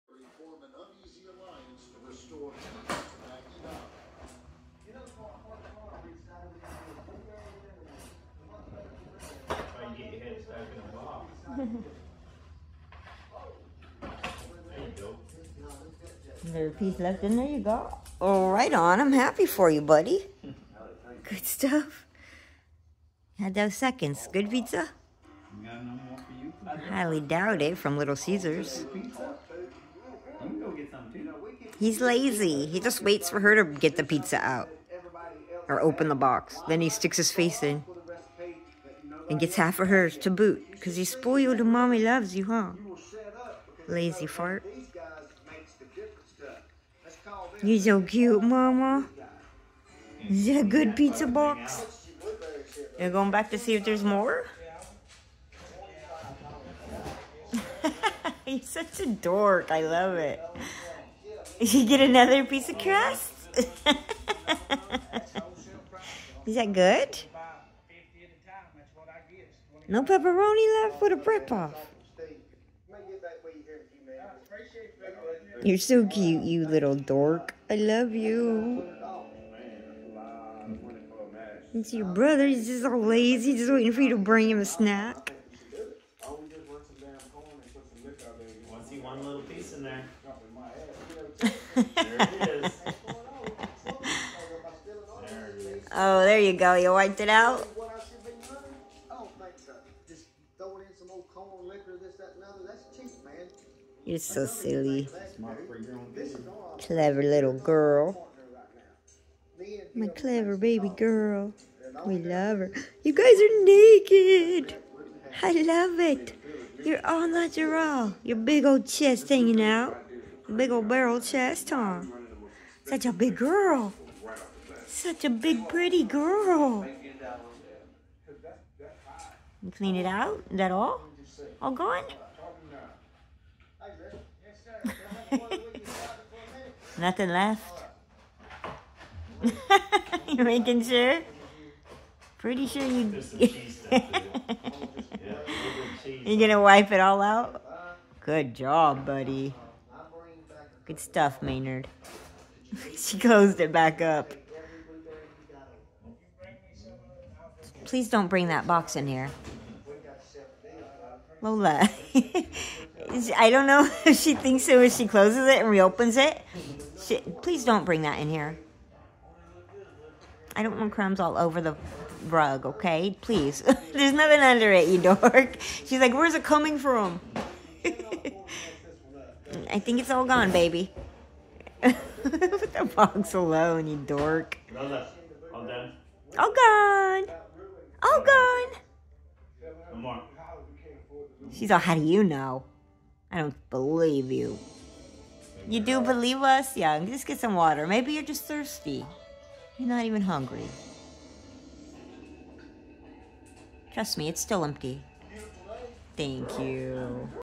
🤭Lola & The Pizza Box🤣
Give a dog a pizza slice and they eat for a day. Give a dog a pizza box with leftover crusts, you get hilarious entertainment 💜